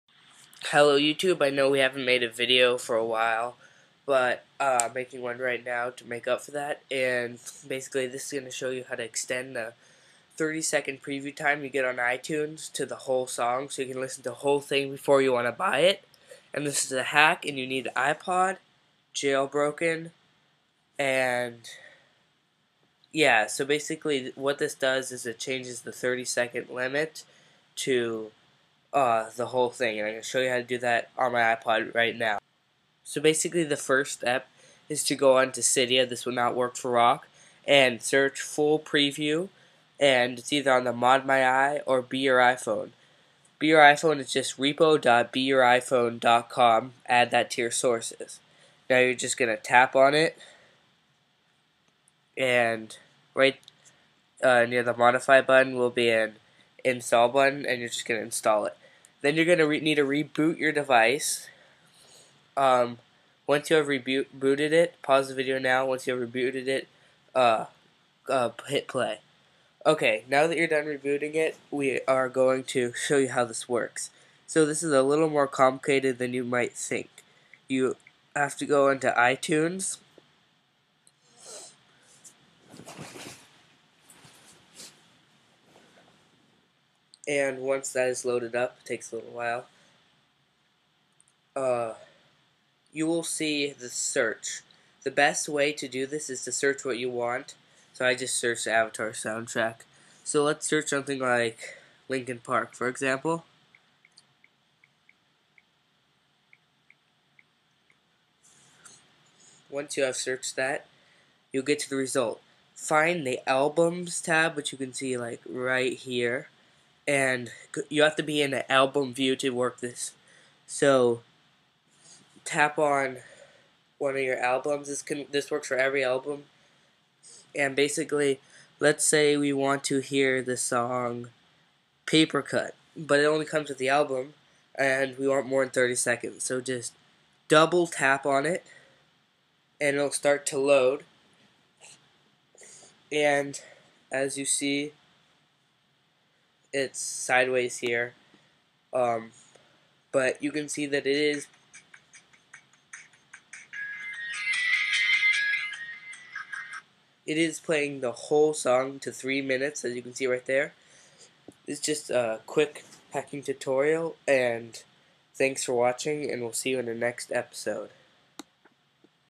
*iTunes Hack: Get ANY Song Free!* (This is legal see description)
Most Recent Jailbreak Tutorial & Download!

This application finds videos to stream of the item you selected. Everything is on a hosting website such as youtube. It doesn't actually hack itunes, but it is still very cool.

Step 1: Get fullpreview
Step 2: Double-tap on a song or video in album mode